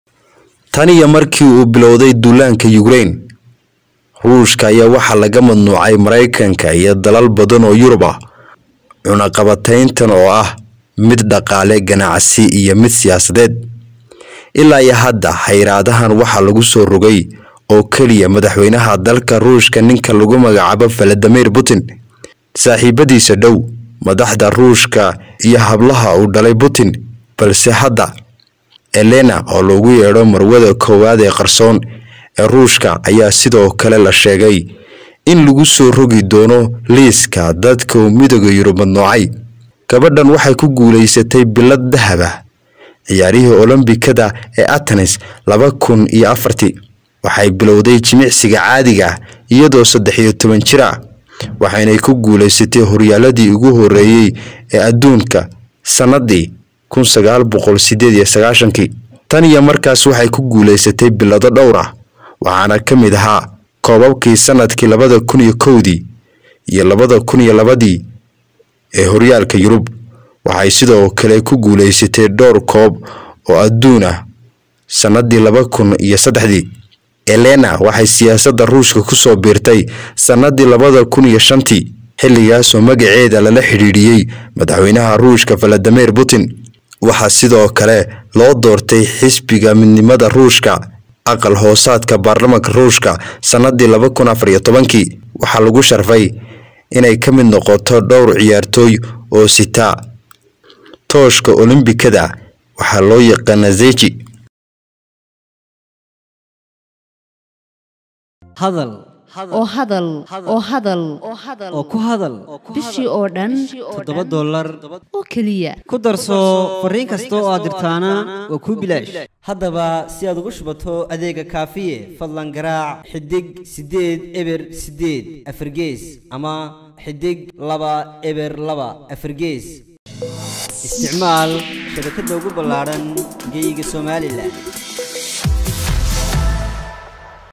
Tallaabada Laga Cabsi Qabo In Midowga Yurub Uu Ka Qaado Marwada Koobaad Ee U Qarsoon Putin
Tallaabada  Laga Cabsi Qabo In Midowga Yurub Uu Ka Qaado Marwada Koobaad Ee U Qarsoon Madaxweynaha Ruushka.